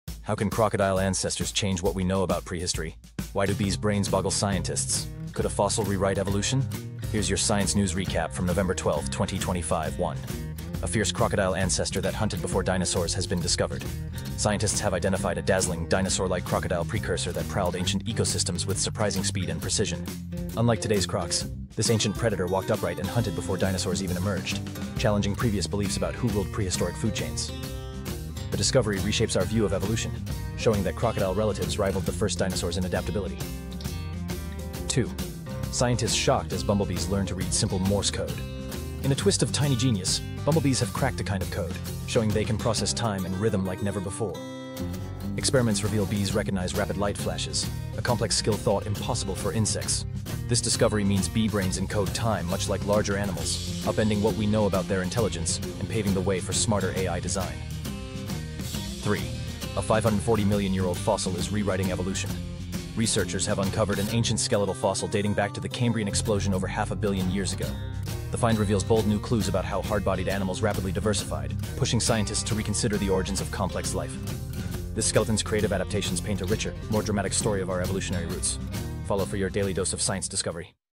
Could a crocodile ancestor change our view of evolution? | Science Recap - Nov 12, 2025
Science News Recap - Nov 12, 2025